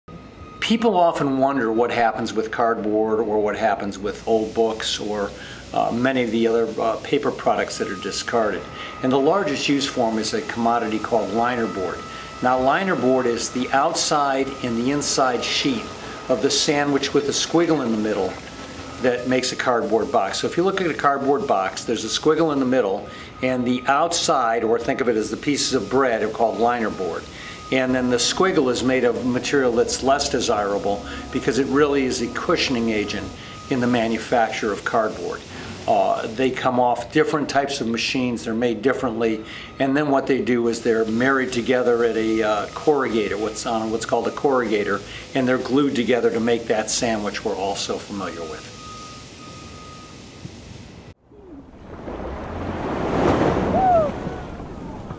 Recycling Books and Paper
In this video, you can see how we shred books into paper that is then recycled and made into liner board. Several types of scrap paper can be used to make new paper products. For years, tons of outdated legal documents were used to make toilet paper.